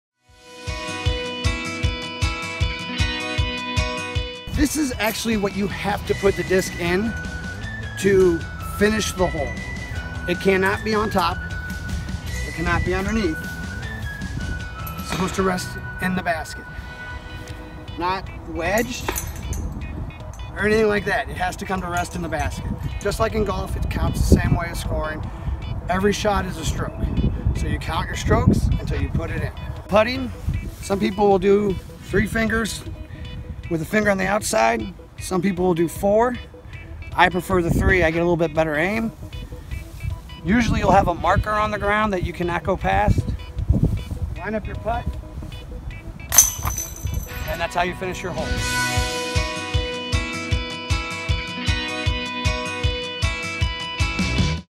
SCCMore: Disc Golf 101 - How to Score
See more of St. Charles County! Tim Wiegard with the St. Charles County Disc Golf Club shares a few tips about disc golf at Quail Ridge Park.